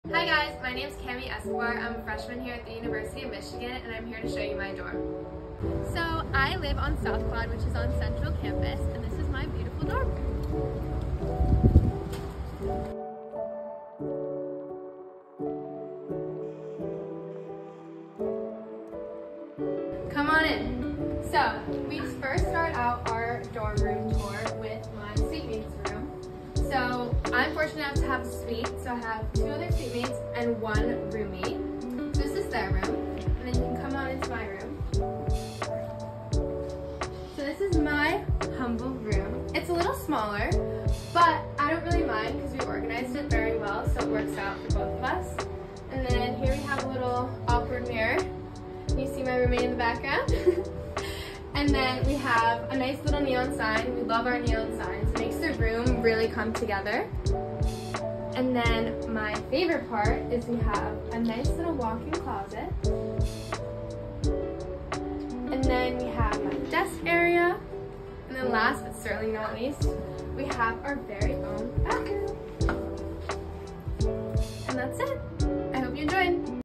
University of Michigan Dorm Tour
Meet Cami, a freshman at the University of Michigan 👋

Want to know what college dorm rooms **really** look like?? Our newest dorm tour series gives you an insider’s look at real dorms rooms across the country. want to film a tour of your dorm? send us a dm on instagram @ dormify 💙💚